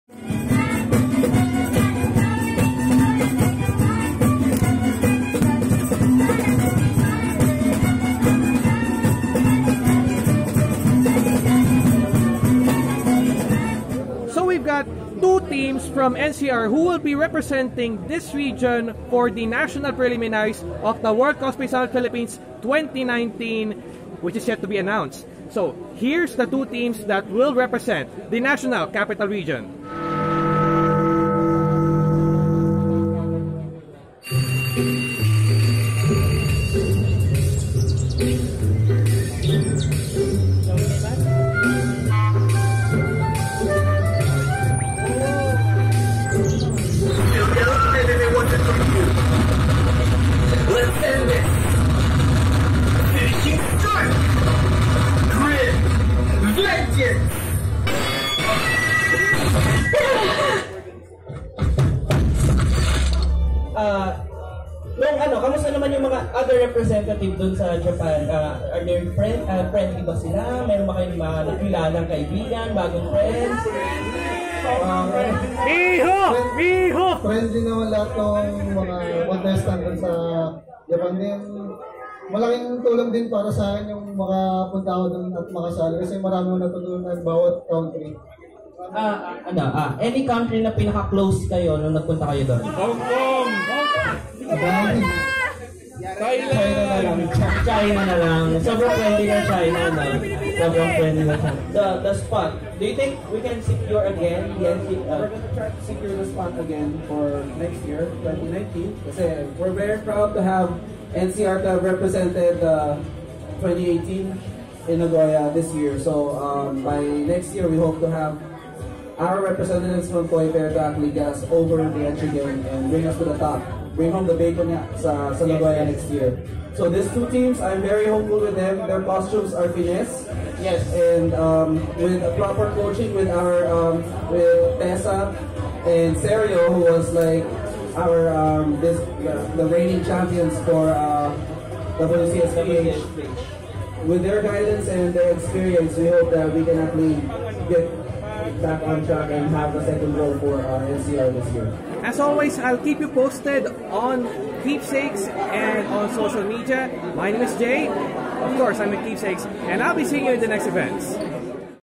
WCSPH2019: NCR Qualifiers at Christmas Toy Fair 2018
The road to World Cosplay Summit Philippines 2019 continues as the NCR Preliminaries has been held at the Christmas Toys and Collectibles Fair 2018 last Sunday, December 16th.